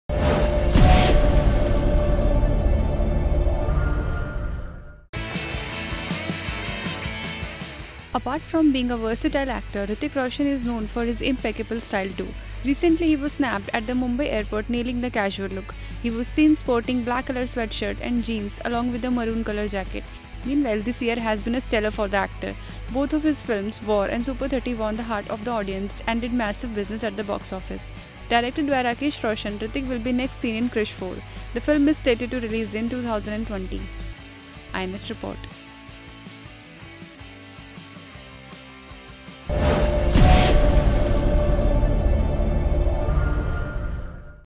Hrithik sports casual look with a cool jacket for his flight
Apart from being a versatile actor Hritik Roshan is known for his impeccable style too. Recently he was snapped at the Mumbai airport nailing the casual look. He was seen sporting black colour sweatshirt and jeans along with a maroon colour jacket.